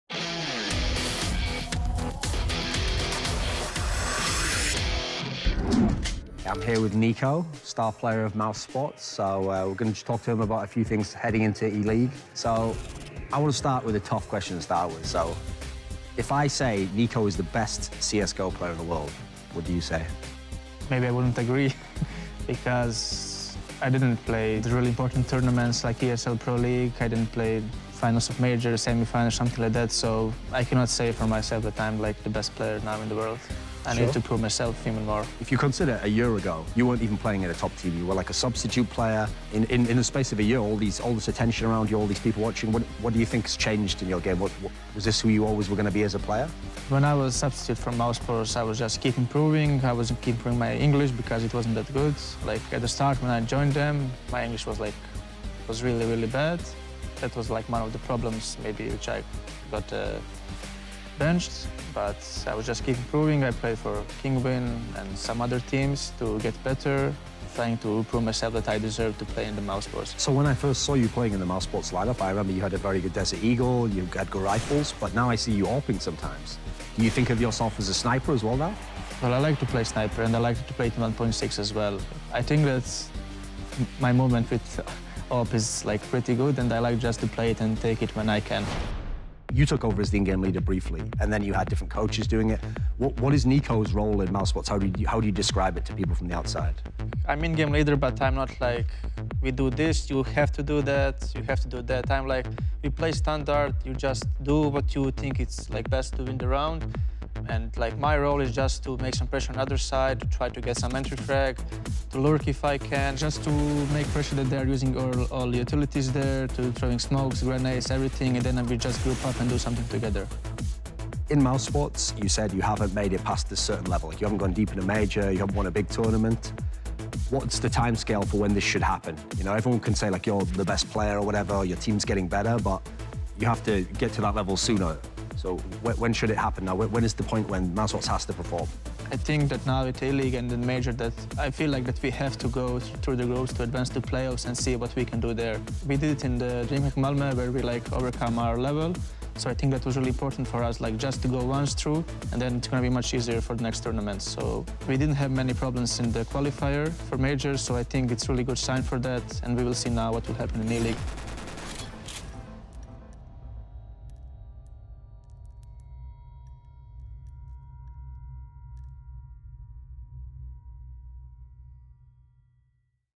NiKo | 1-on-1 Interview | CS:GO S1
Nikola “NiKo” Kovač sits down with Thorin to discuss his rise to the top, his perspective on being in-game leader and where he envisions mousesports going in the future.